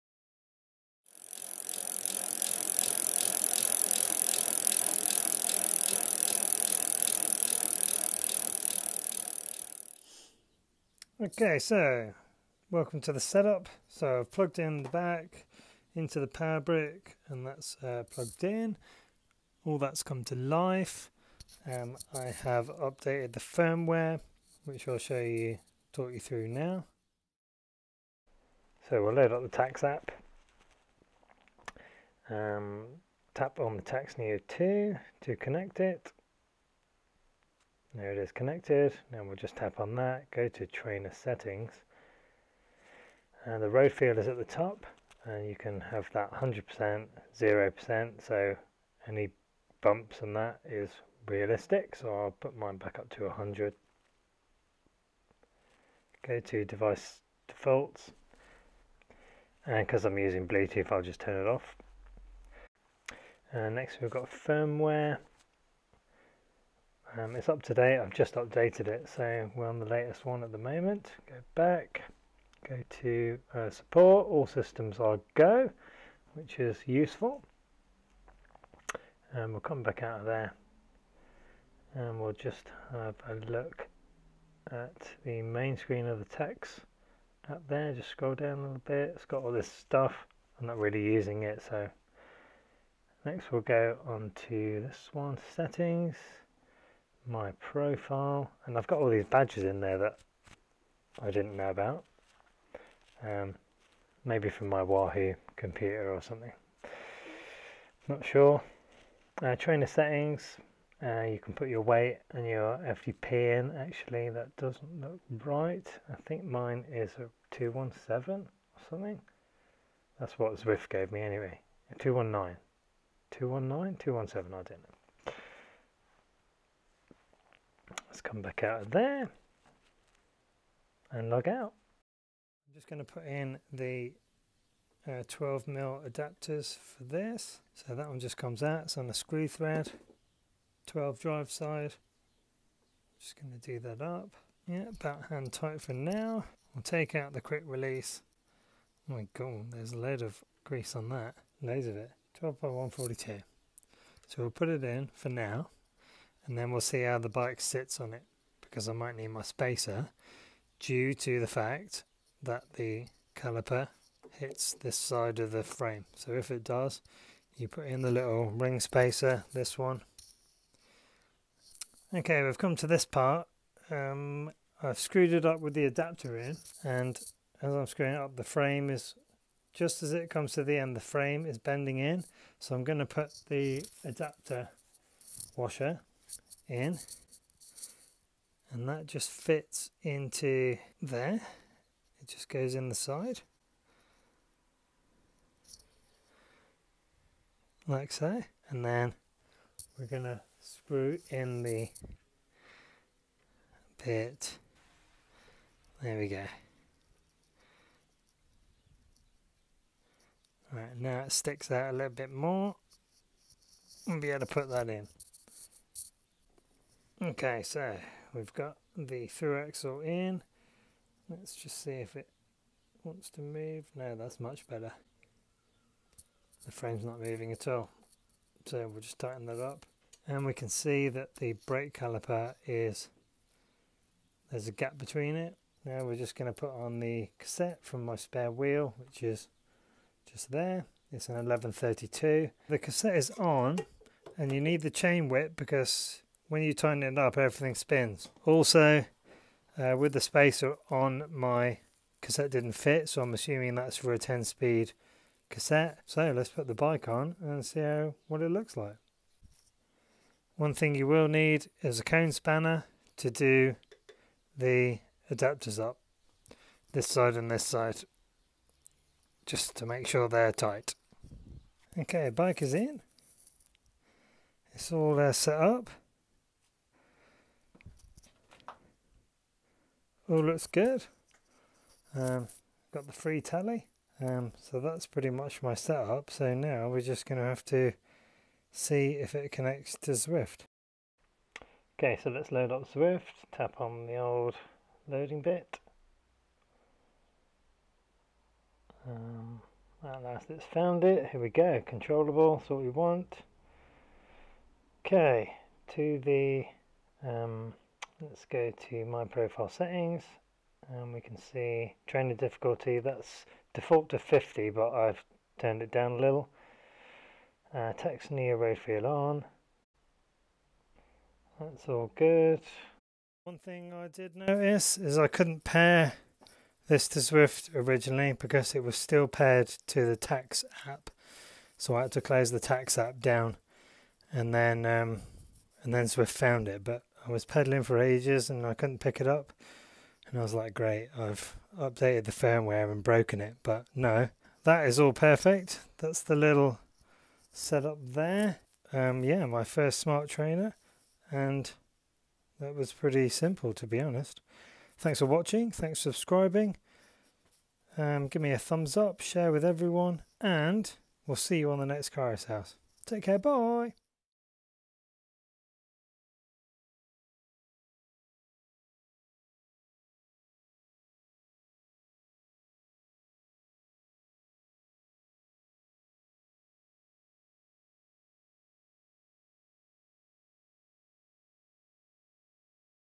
Tacx Neo 2 Setup Guide
In this video, I go through step by step how I set up my Tacx Neo 2.

We set up the tacx app and then zwift.

Put the cassette and bike onto the trainer using the correct adapters.